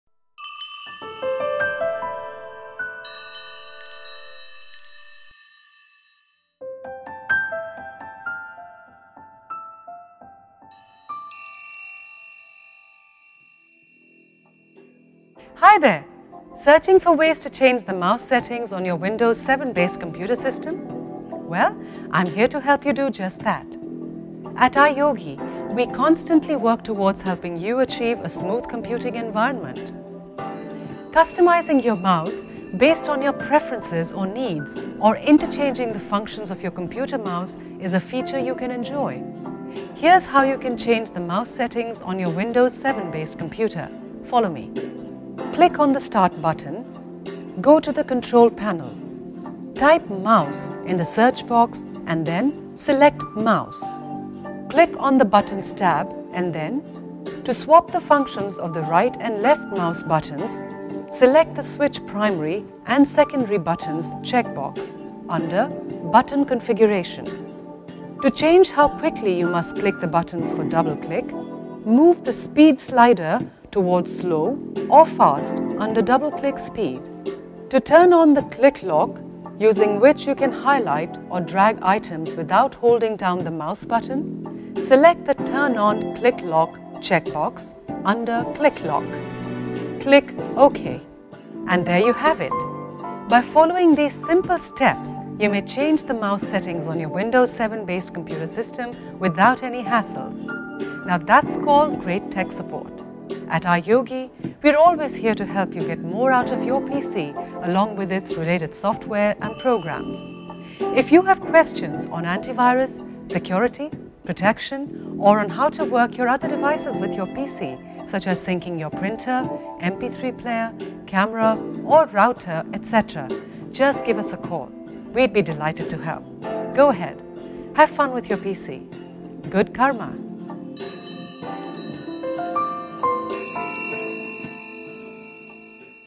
How to change mouse settings on Windows® 7 based pc
Do you wish to change the mouse settings on your Windows® 7 based PC?  Follow the steps shown in the video .